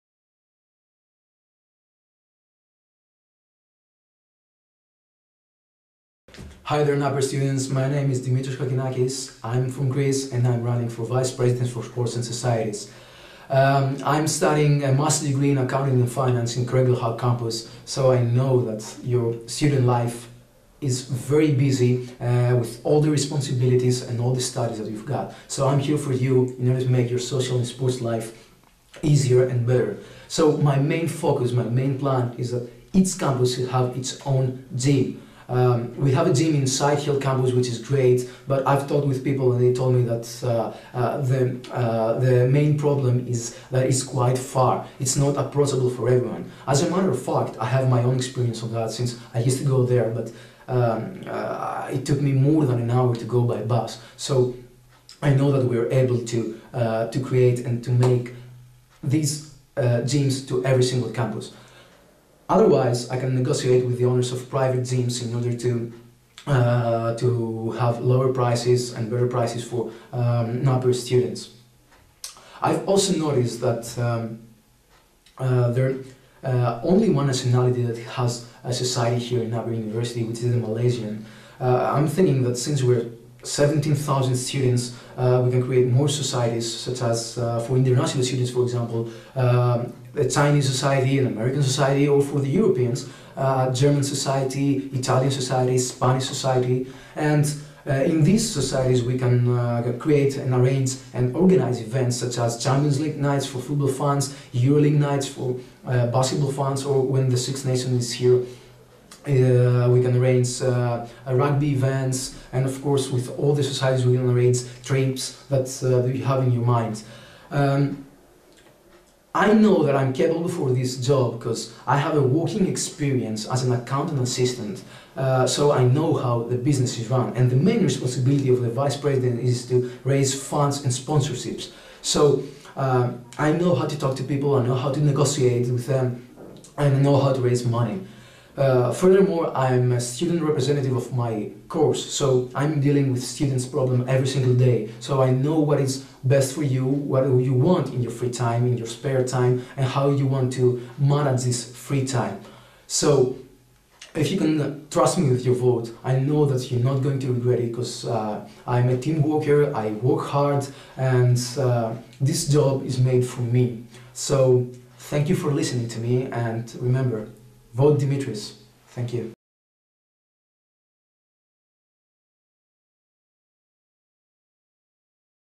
Dimitris Kokkinakis - candidate for NSA Vice President Sports & Socs 2013
Dimitris Kokkinakis for NSA Vice President Sports & Socs President.

NSA Student Elections 2013 - Vote via the student portal now - Monday 18th - Friday 22nd March.